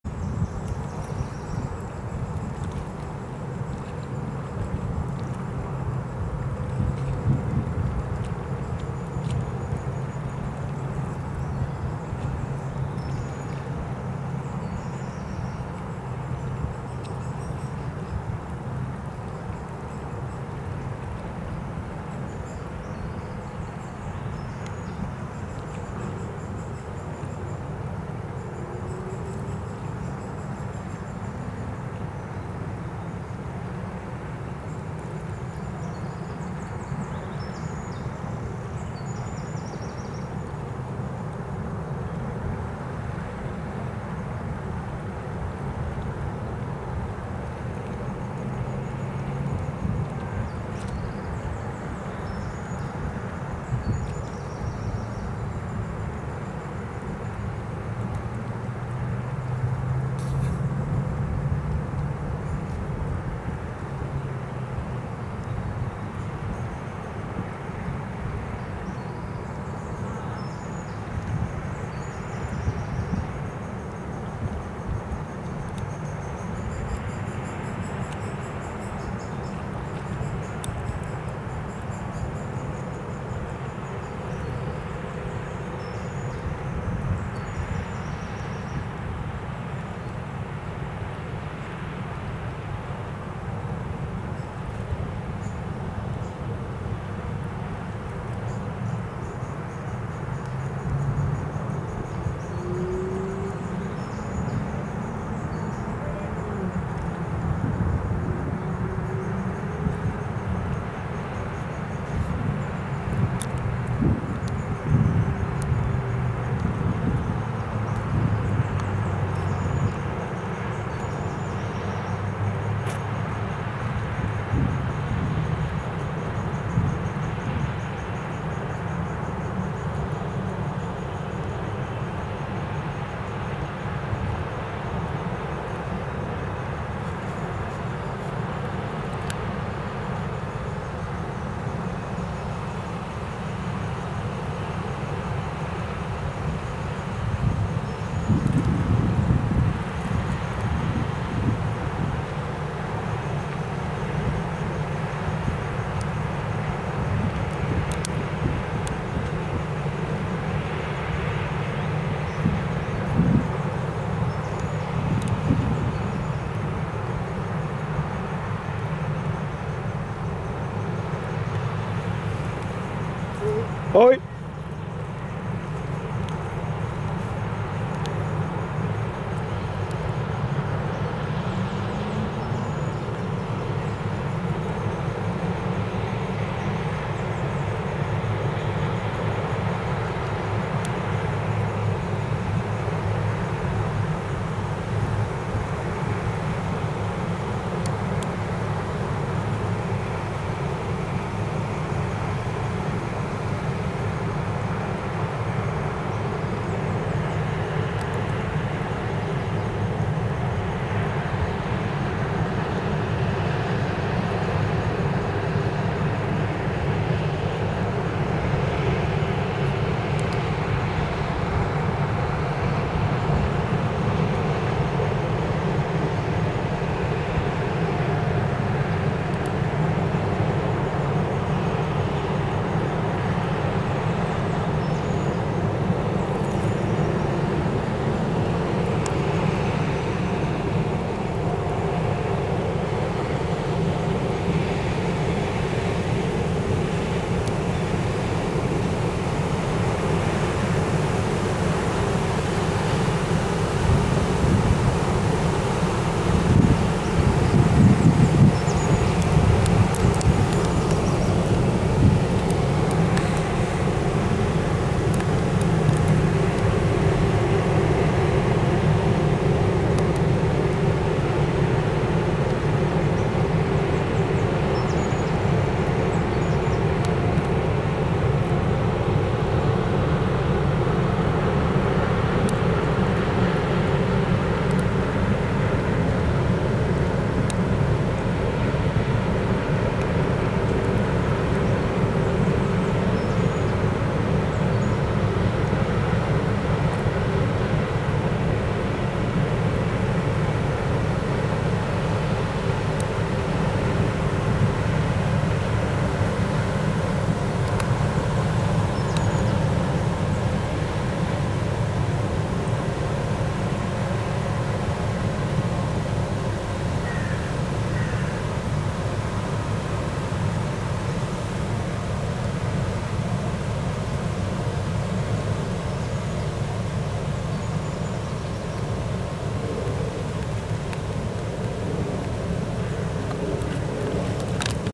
ICARUS - IMO 9259240
Photo Category: Inland Tankers
Description:
Arriving below locks at Maasbracht/Limburg (The Netherlands) during southbound transit.
Length 86m beam 11,40m draught 3,35m
Tonnage : 2010 tons
Home port : Ermelo (NL)
Operated by Wijgula Shipping at Druten (NL)
Main engine : ABC 6 MDZC (1500hp)
Callsign :PF 4093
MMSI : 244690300
Built in 2002 by shipyard Ceske Lodenice at Usti/CZ (casco) and completed by shipyard Smits at Krimpen a/d Lek (NL)
ENI 02325626

 Vessel Identification

Name: Icarus
IMO: 9259240
Flag: Netherlands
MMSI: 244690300
Callsign: PF4093
ENI no.: 02325626

 Technical Data
Vessel type: Inland Tanker
Gross tonnage: 1,606 tons
Summer DWT: 2,045 tons

 Additional Information
Home port: Ermelo